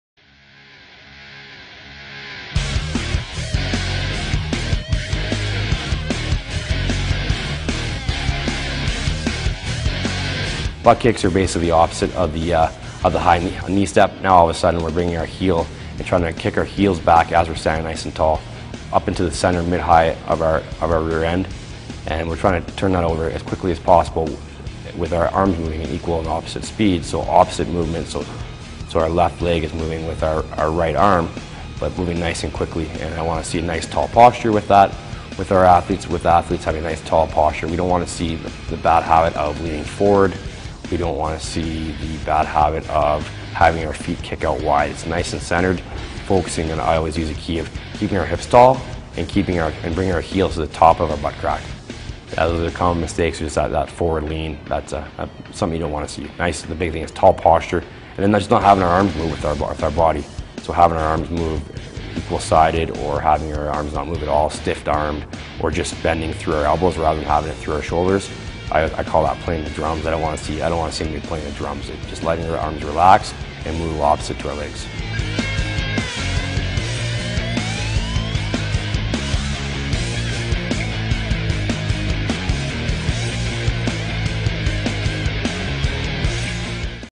Butt Kicks featuring Duncan Keith
Crash Conditioning workout series for elite hockey players featuring Duncan Keith. Video production by STACK